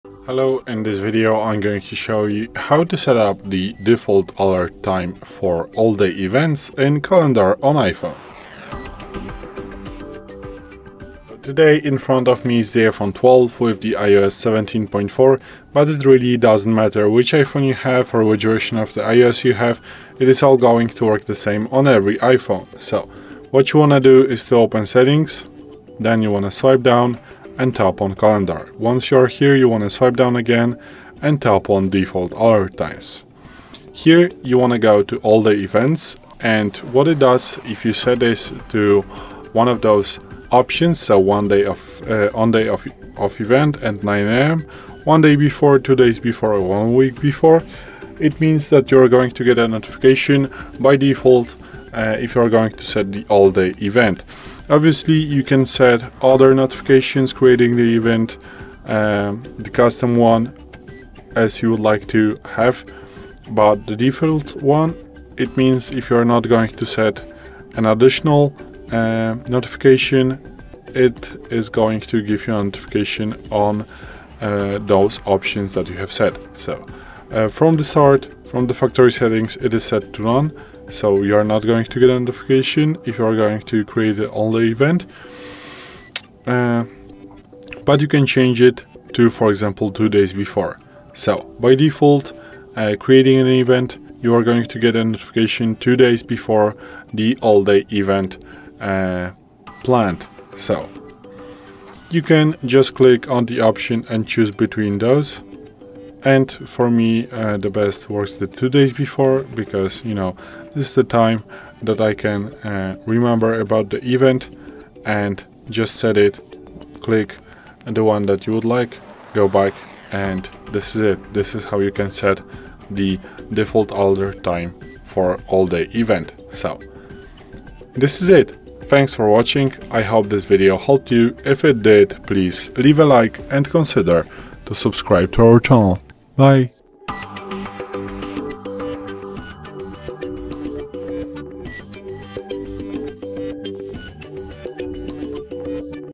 How To Set Default Alert Notification Time for All Day Events in Calendar on iPhone
Discover how to customize default alert notification times for all-day events in the Calendar app on your iPhone. Setting up default alerts ensures you never miss important reminders or appointments. Follow our simple guide to personalize your calendar notifications and stay organized effortlessly.

Why is it important to customize default alert notification times for all-day events on your iPhone?
How do you access and adjust default alert settings in the Calendar app?
What are the recommended alert times for different types of all-day events?
How can setting default alerts improve your productivity and time management?
Are there any tips for managing notifications effectively in the Calendar app?